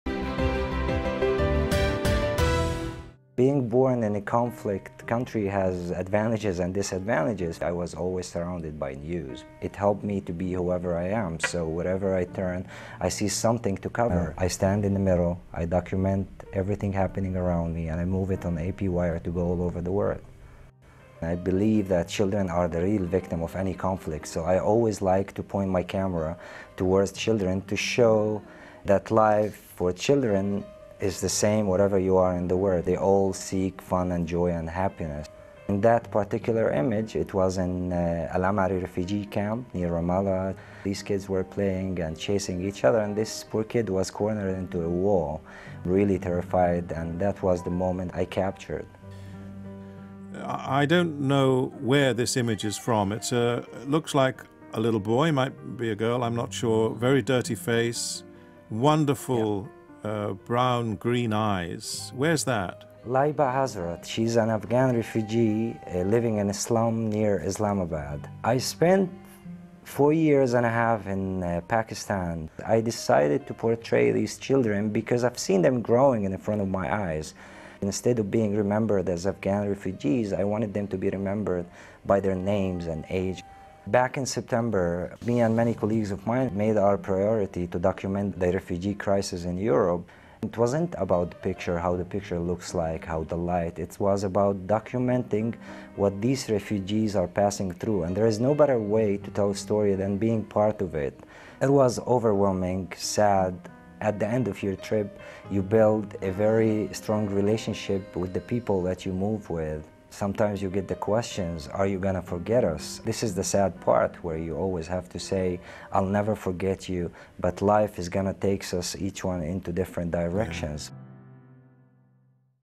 Standing in the middle, photographing conflict - BBC News
Jerusalem-born photographer Muhammed Muheisen grew up with news, keeping a camera around his neck. Early on, he learned a precious lesson: to photograph conflict, you have to position yourself in the middle. As AP's Chief Photographer for the Middle East, Afghanistan and Pakistan he has been roaming the region for the past decade, photographing wars and the people caught up in them. His work has received numerous awards, among others, Breaking News Pulitzer Prize winner in 2005 and 2013 for covering the war in Iraq and the civil war in Syria.

(Image: Afghan refugee, Laiba Hazrat. All photos by Muhammed Muheisen, Asociated Press)